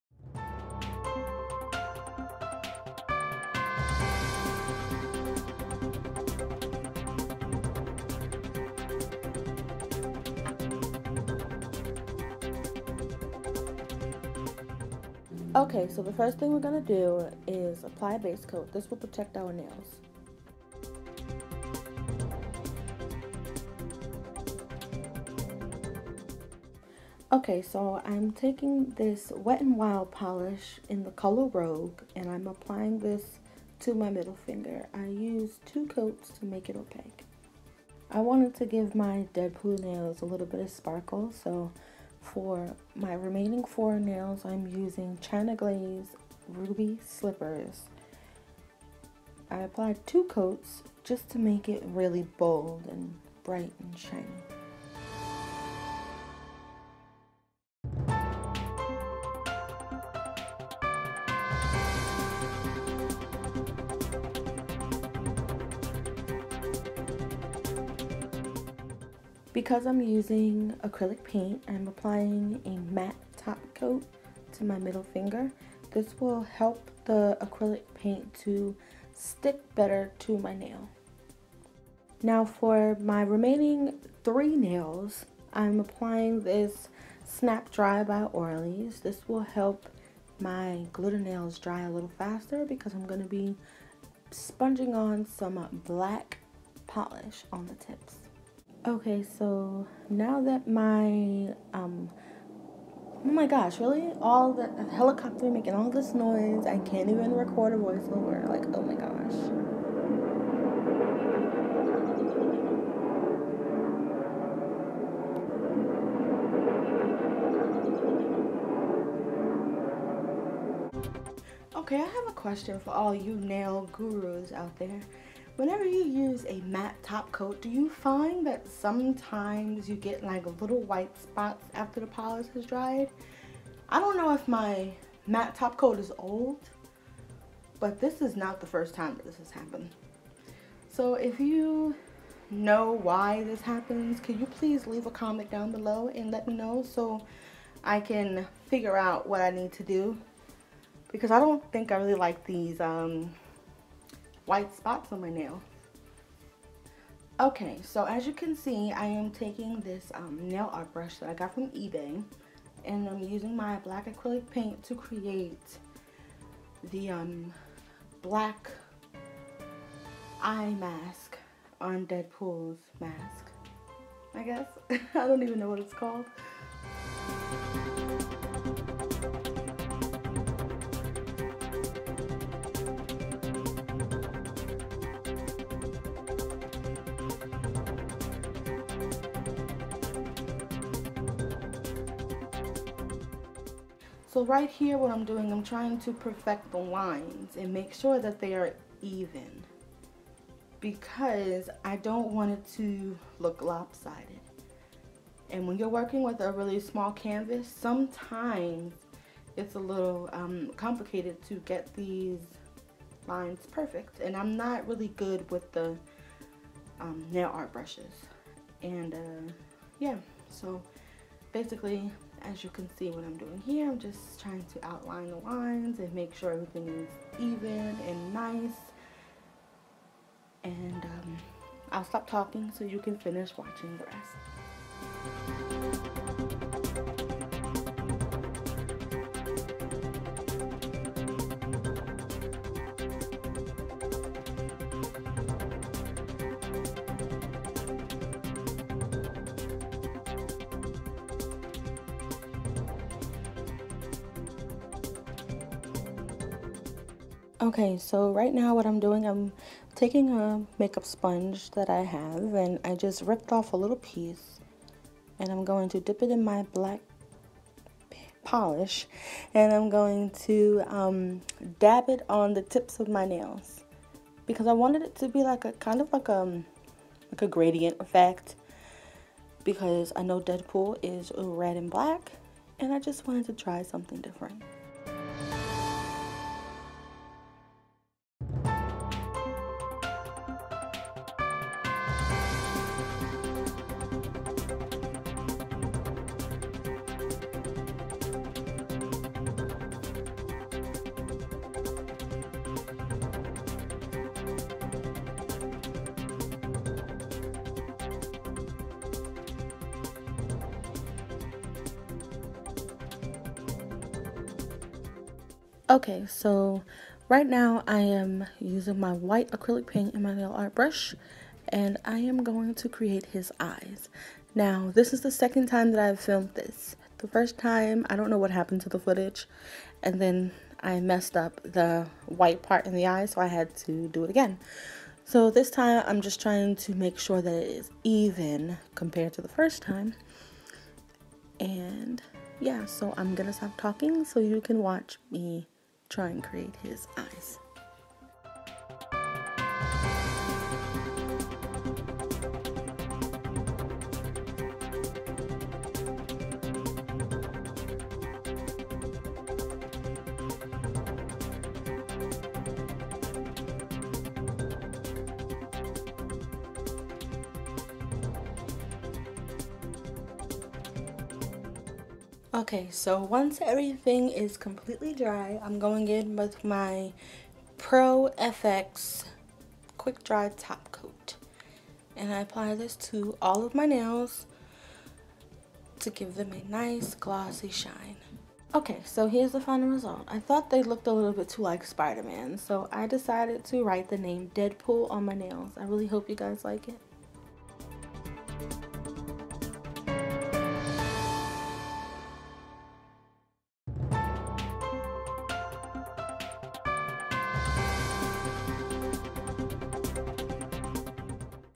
DeadPool | Inspired | Nail Art
Hello guys in today's video I'll be do an inspired DeadPool nail art design I really hope you like it!!!

Products Used...
Seche Clear - Base Coat
Wet n Wild - Rogue
China Glaze - Ruby Slippers
Apple Barrel Acrylic Paint
•Black
•White
Pro FX - Quick Dry Top Coat

Nails 💅🏽 & Horror 👻